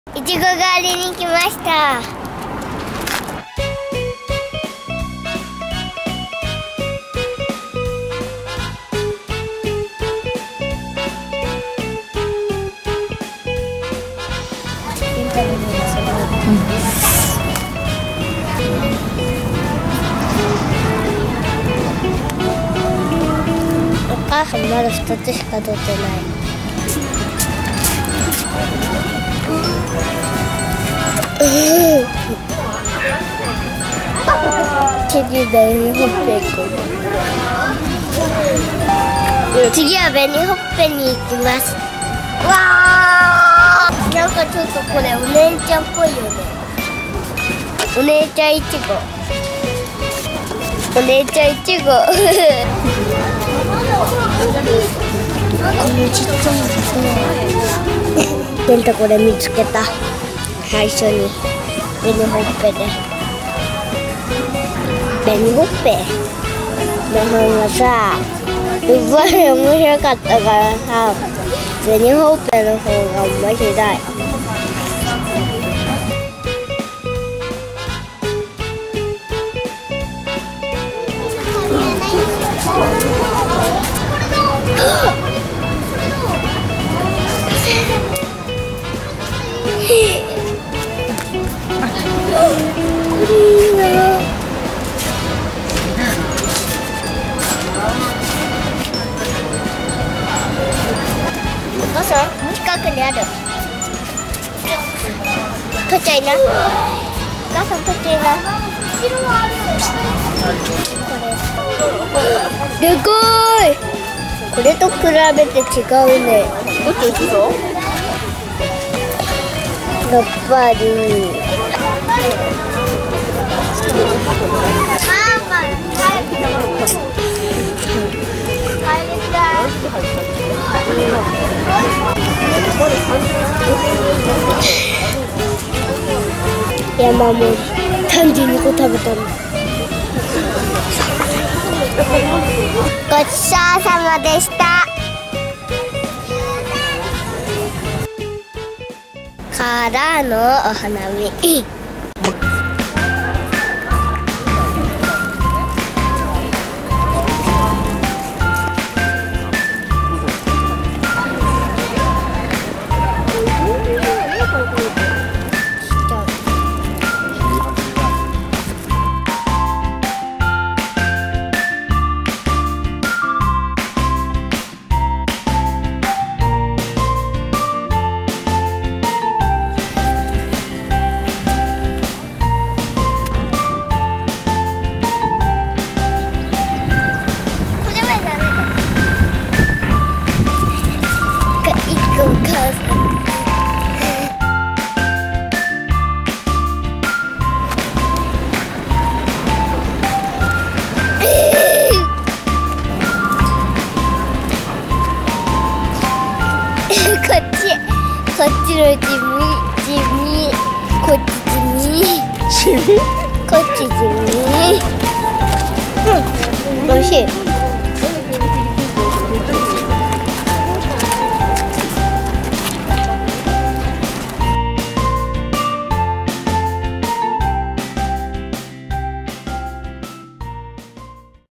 いちごがり＆おはなみに行ってきました♪
初めてのいちご狩り、たくさんたべてげんたは大満足でした！